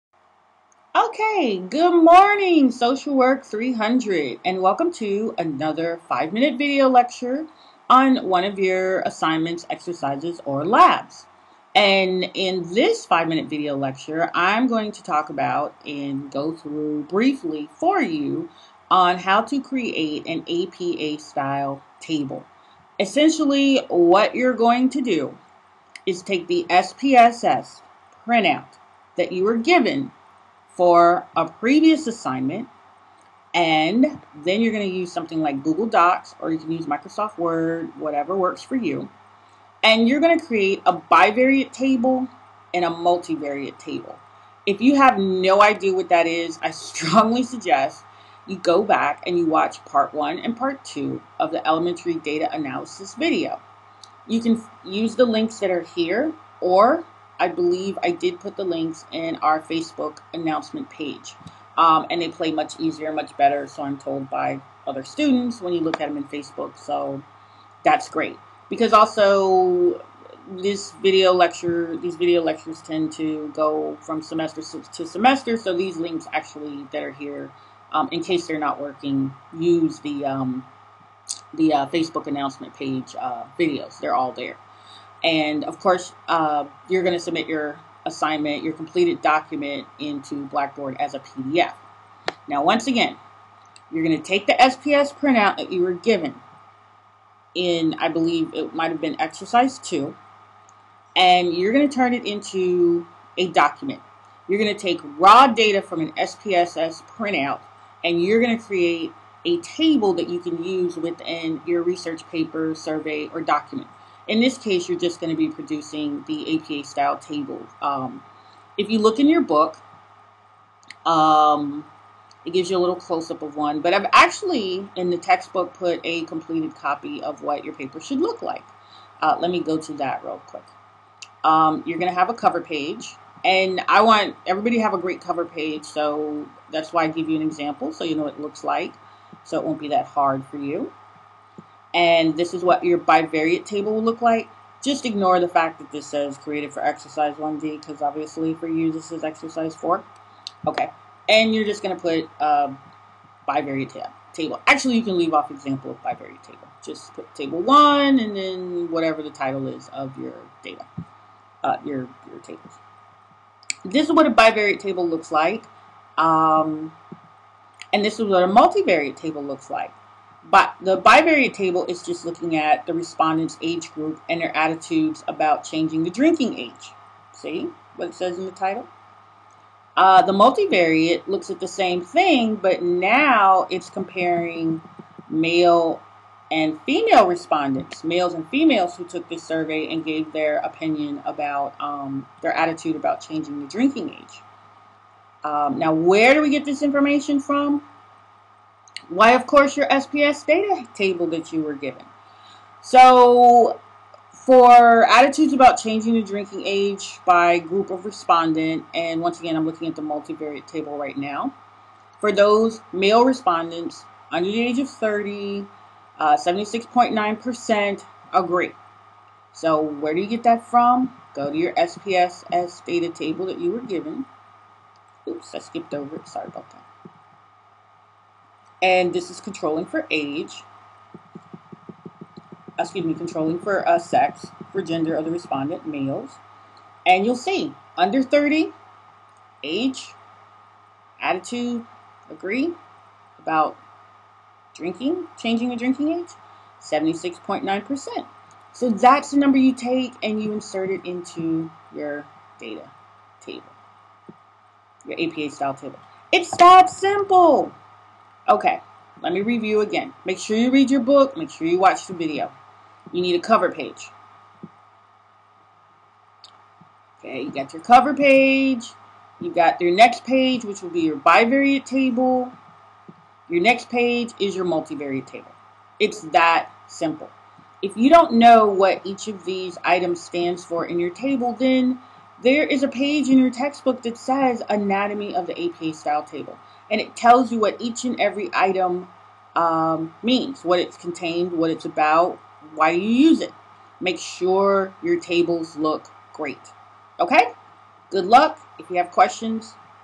Apa Style Data Table
REMEMBER TO FOLLOW THE COMPLETE INSTRUCTIONS IN YOUR COURSE TEXTBOOK

Based on the unique SPSS printouts that you will be given in class, use GOOGLE DOCS to produce a bivariate table and a multivariate table.

Each student will be given a copy of an unique SPSS printout. (SPSS Printouts are located in the APPENDIX)  Each contains a bivariate crosstab and a multivariate crosstab.  In this course, you will soon produce SPSS crosstabs just like these.  You will be required to present the data in the crosstabs according to APA Style format.  This is the best format for presenting numerical tables in word-processing documents.